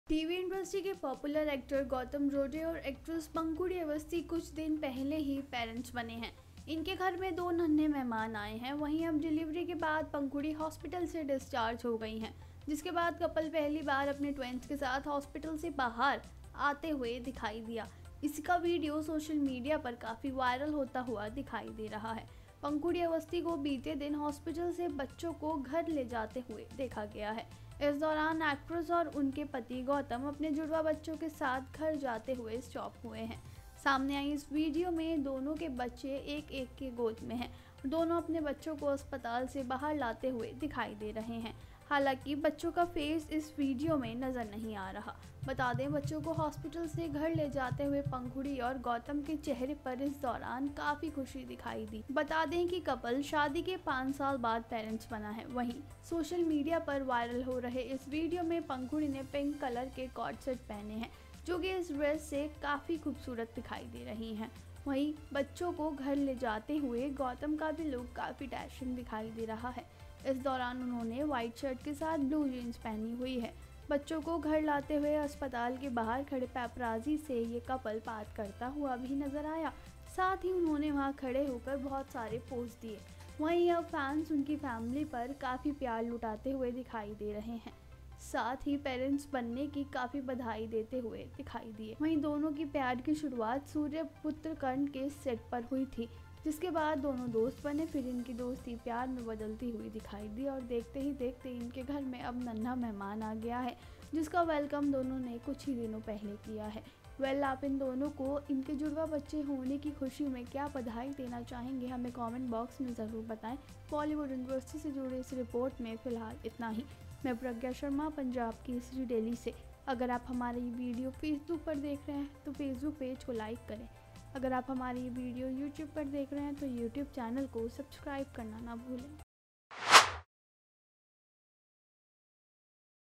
हॉस्पिटल से डिस्चार्ज हुई Pankhuri Awasthy, सामने आई बच्चों की पहली झलक
TV industry's popular actor Gautam Rode and actress Pankhuri Awasthi have become parents a few days ago, two little guests have come to their house. Now Pankhuri has been discharged from the hospital after delivery. After which the couple was seen for the first time bringing them out of the hospital with their twins. Its video is seen becoming quite viral on social media.